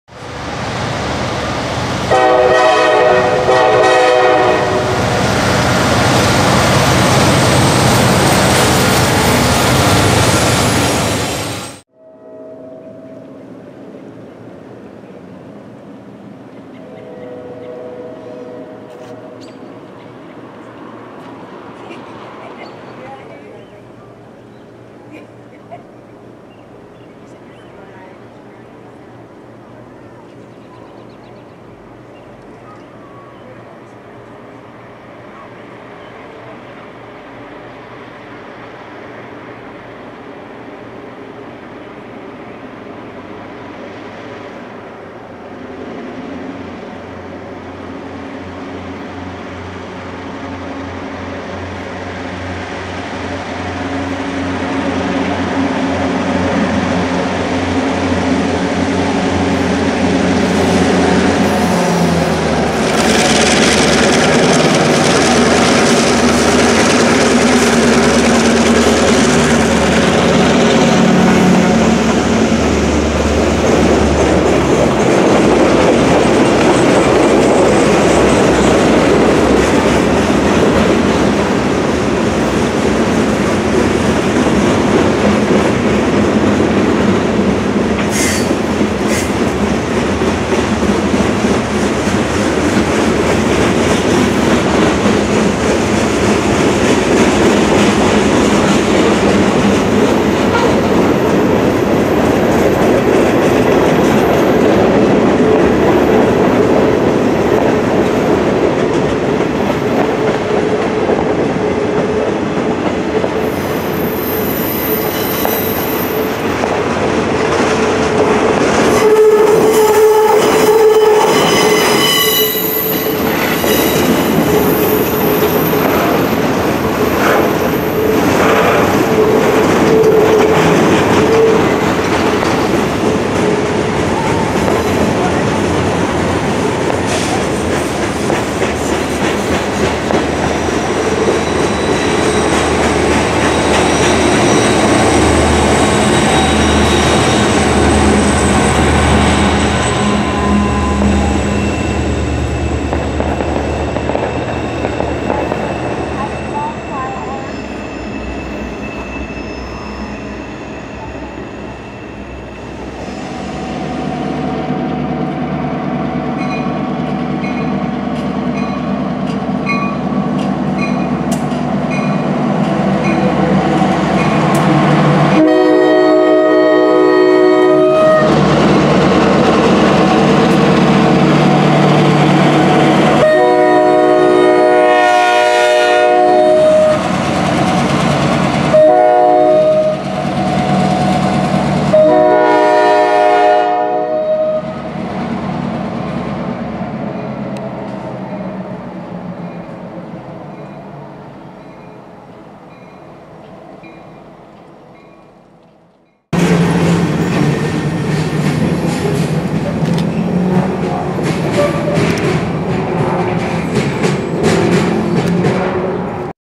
NS Geeps Roaring through Gallitzin, PA w/ Awesome Quilling K5LA Horns!
6/25/18 - Pennsylvania Series 2018 Part 8

An NS local roaring through Gallitzin, PA under the cover of darkness with EMD GP38-2s leading! Also, notice the awesome quilling K5LA on one of the units (@ 2:45).